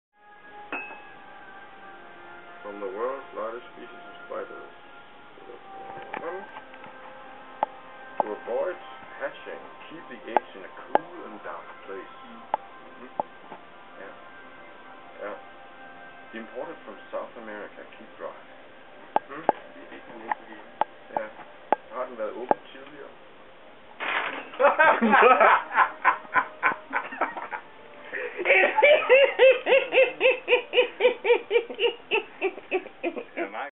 stig opens bag with living spider eggs
stigs horror